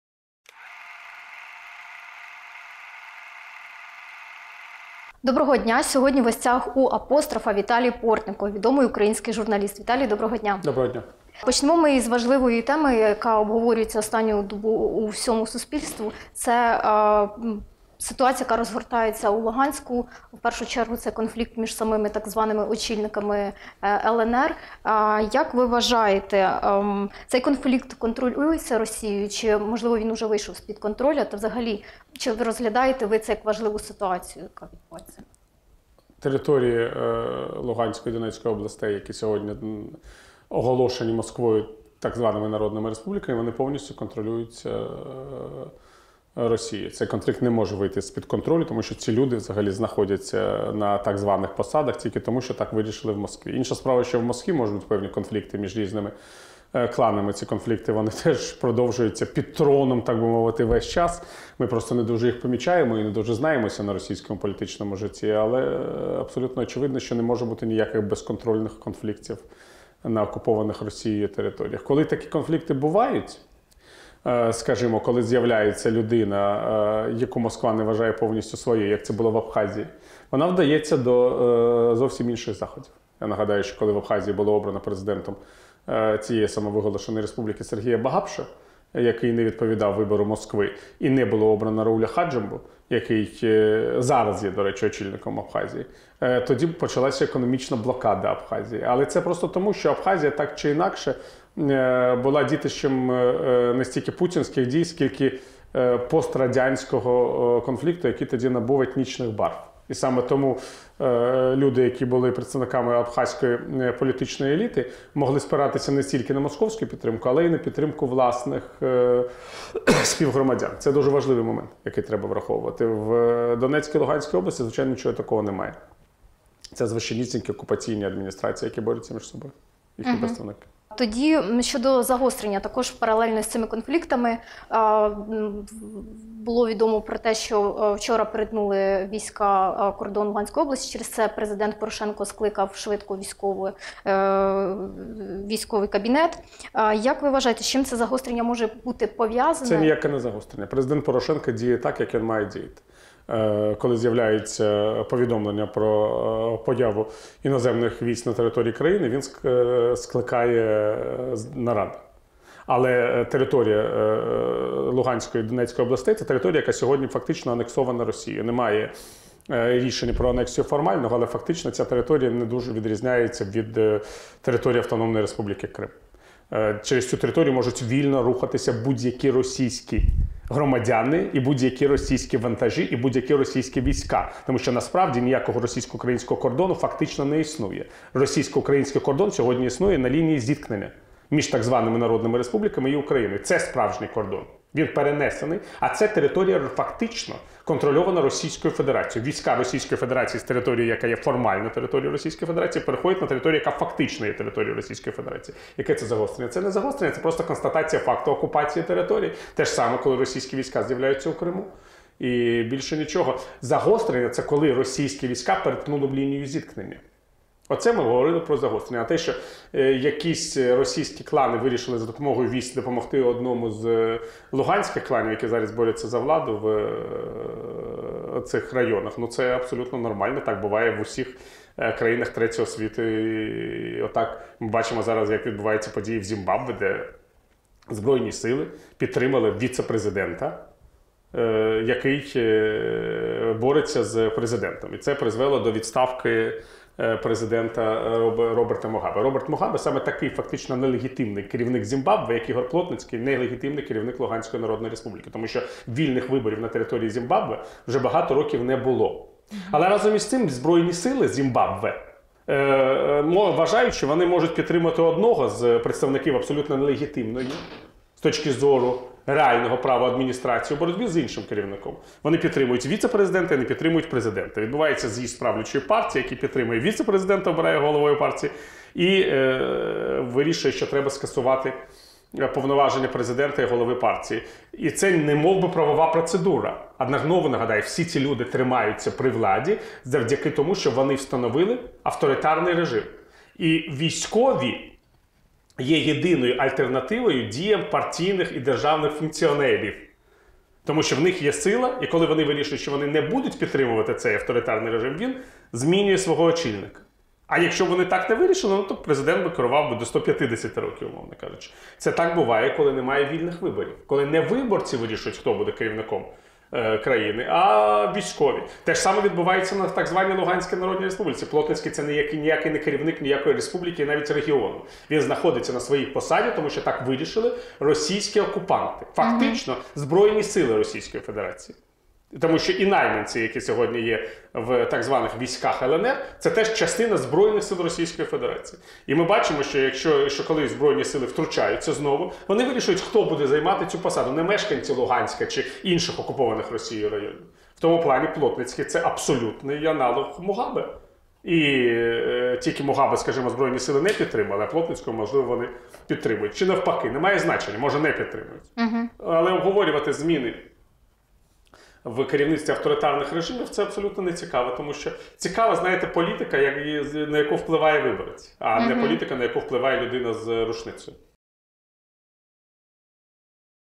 Конфлікт в Луганську повністю контролюється Москвою - Віталій Портников
Журналіст Віталій Портников коментує військовий конфлікт з ознаками перевороту в так званій ЛНР